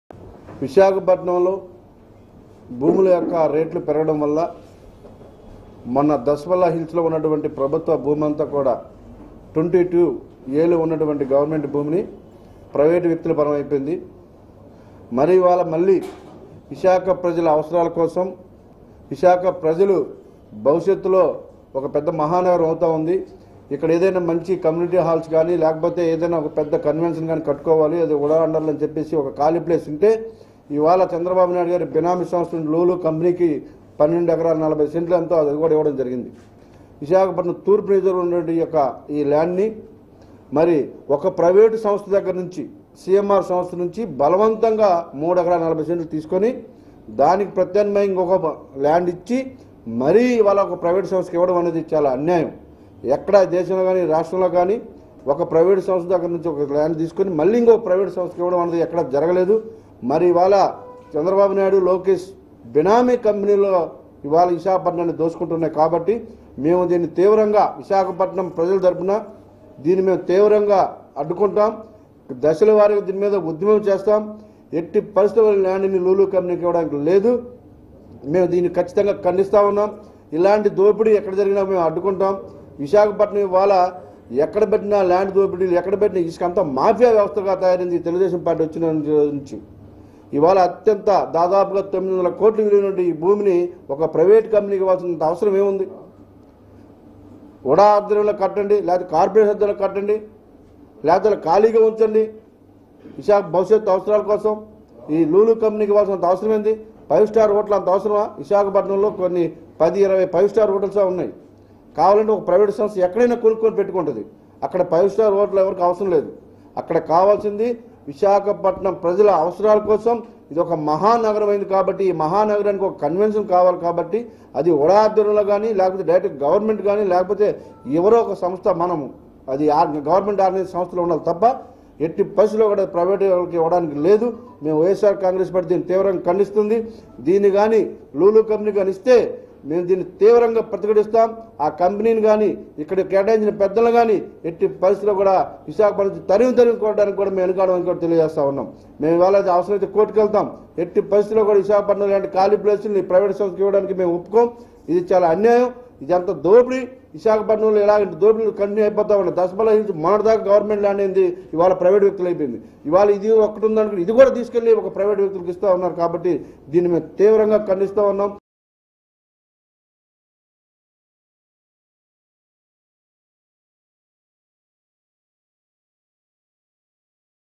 YCP Vamsi Krishna Srinivas Fires on TDP Govt. About Govt. Lands in Vizag || man
YSR Congress Party East Convinor Vamsi Krishna Srinivas Fires on AP Goverment  About Visakhapatnam Govt. Lands.
Visakhapatnam